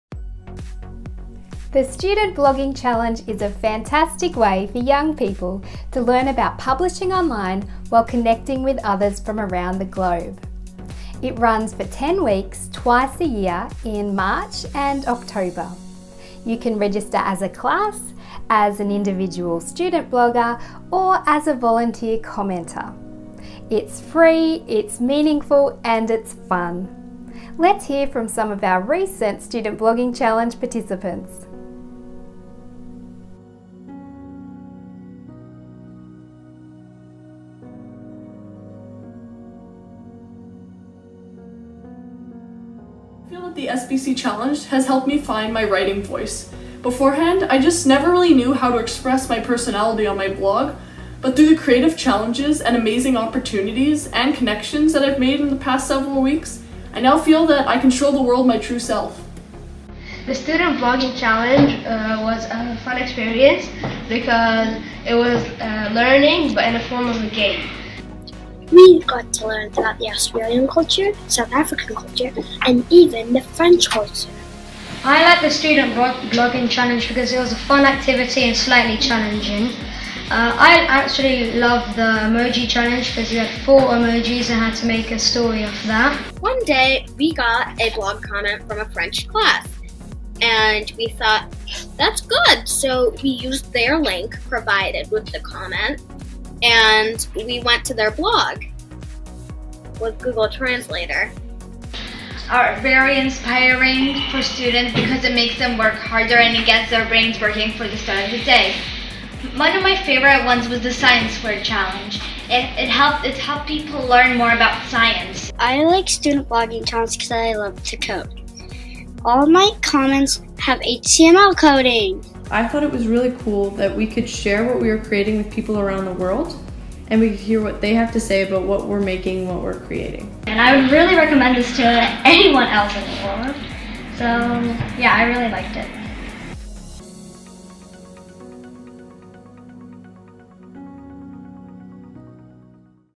Why Take Part In The Student Blogging Challenge?
The Student Blogging Challenge runs for 10 weeks every March and October.

In this video, some past participants share what they got out of the challenge.

Thank you to the students who featured in the video from California (USA), Performance Learning Program at Seycove (Canada), and British International School Belgrade (Serbia).